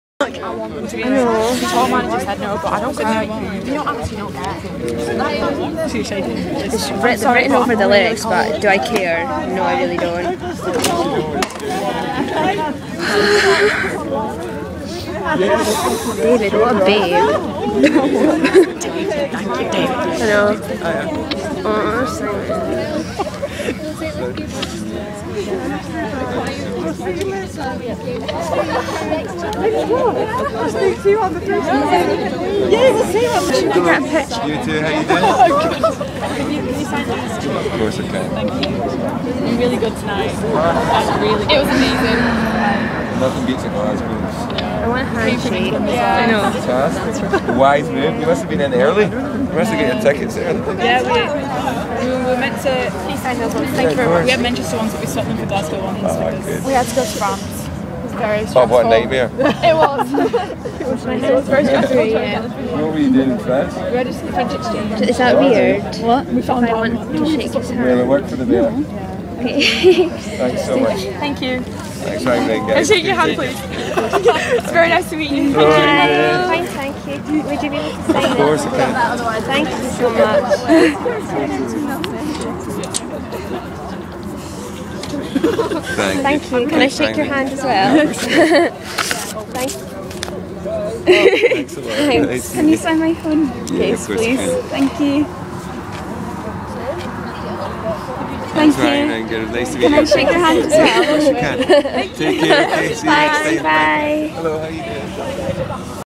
Meeting Simon Neil from Biffy Clyro - SECC 1/4/13
A really crappy ipod video of my friend and I meeting Simon Neil outside the SECC after the Biffy Clyro gig. We start speaking to him properly at 1:15 - a short and sweet "hello and cheerio" but it was still the best conversation of my life so far. You can't really see much cos I'm a numpty and was too scared to ask if I could point the camera at his face, but you can see his shoes and his trench coat and hear his beautiful wee voice... I sound like a right obnoxious weirdo when I'm rambling on in the video, so just ignore me. I had a cold and I was dying of happiness - I think I can be excused... ;) We met Ben and James Johnston as well - they were so nice and they talked to us for ages but my ipod had a fit and wouldn't let me record it. Special thanks to David Leitch, @dryburgh_soul on twitter, for helping us meet them... "whatta babe" - 0:15 then you can hear Olivia muttering "David, thank you David" ahaha...

I MENTIONED HANDSHAKE FIRST :(